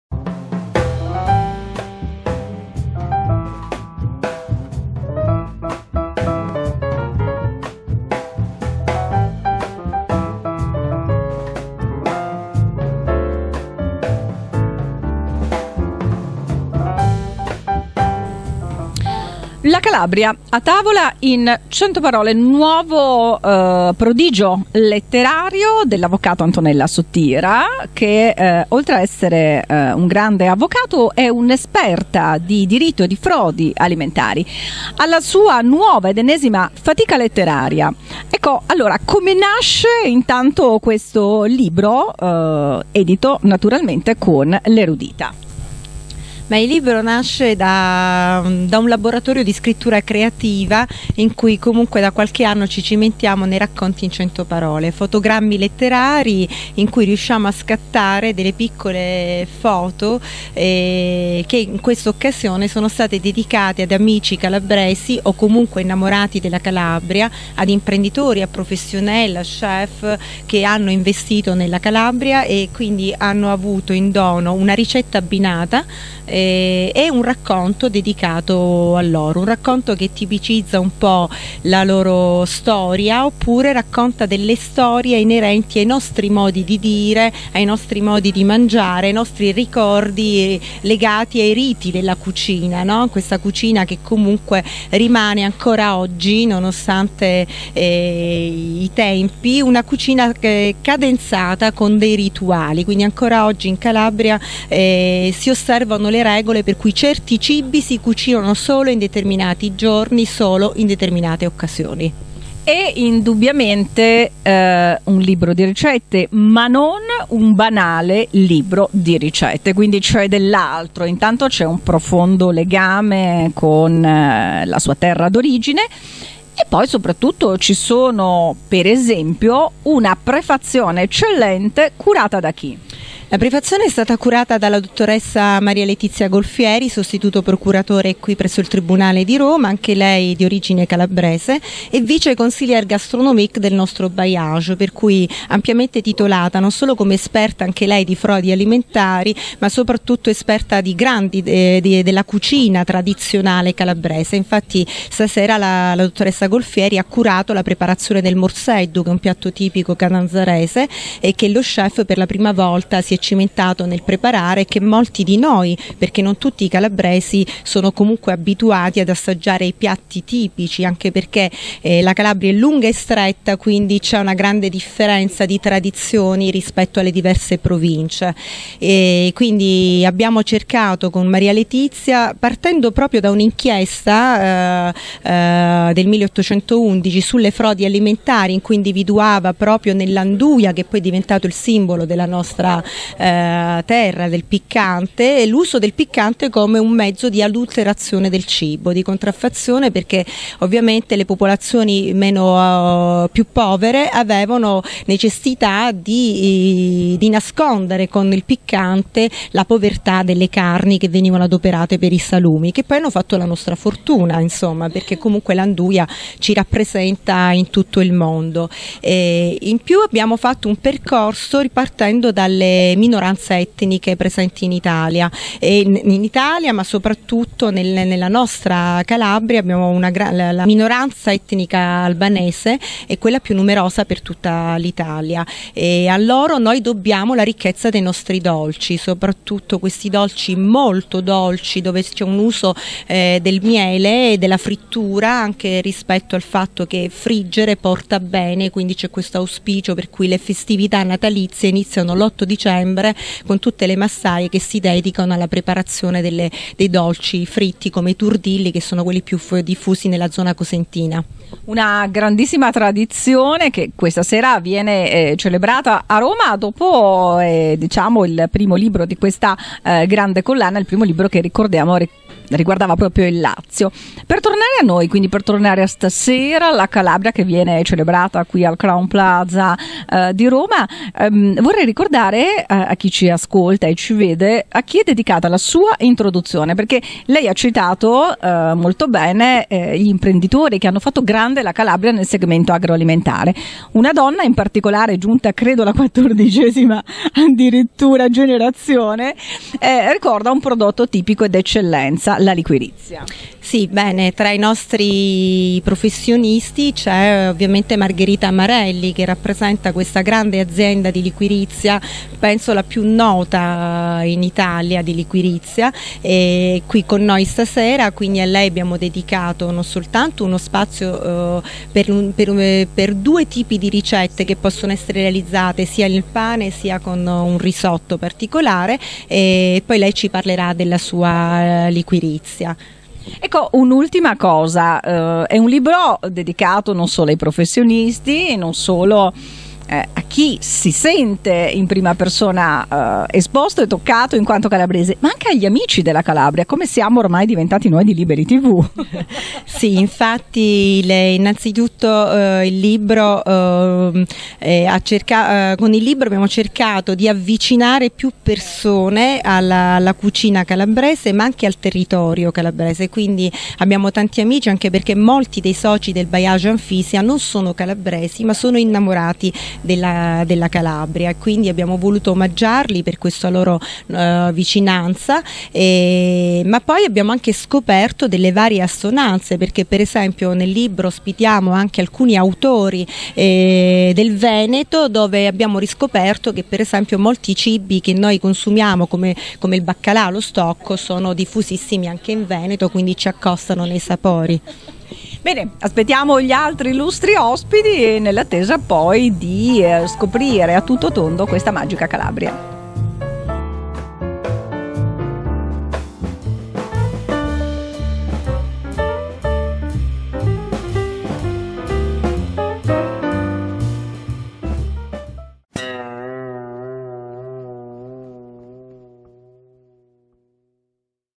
Antonella Sotira - Presentazione del libro "La Calabria a Tavola in Cento Parole"
Registrazione effettuata da Liberi.tv il 17/03/2017 presso il Crowne Plaza Rome St. Peter's

Presentazione del libro "La Calabria in Cento Parole" a cura di Antonella Sotira, avvocato e presidente dell'associazione IUS Gustando, edito da L'Erudita

Intevista di CAMILLA NATA alla curatrice del libro ANTONELLA SOTIRA